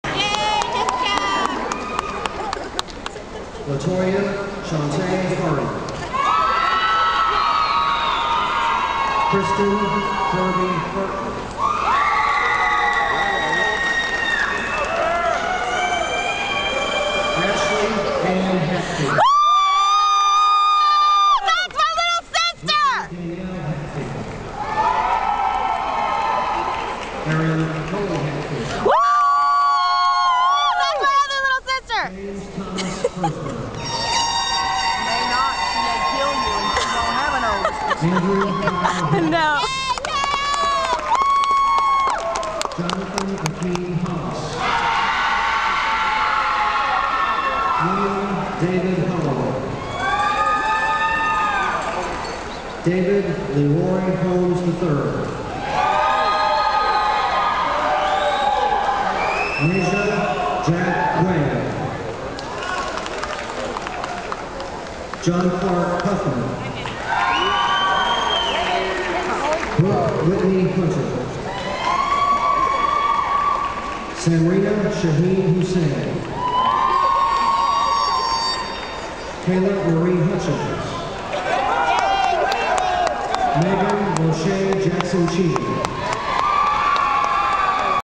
My Little Sisters' Graduation 3
Well, there they go!

You can hear me screaming at them but what's the funniest is that the lady in front of me got kinda peeved, probably because what I said was just so original and she was mad she didn't think about saying something that cool to her grandson.

She tried to do one of those half-hearted joke things where it's not really a joke 'cause the person's really still upset about it but you could tell she thought it up in her mind a little too long before saying because it came out a little retarded.

AWKWARD...for her. Hilarious for me. Stupid old ladies that think the world should bow down to only THEIR grandchildren.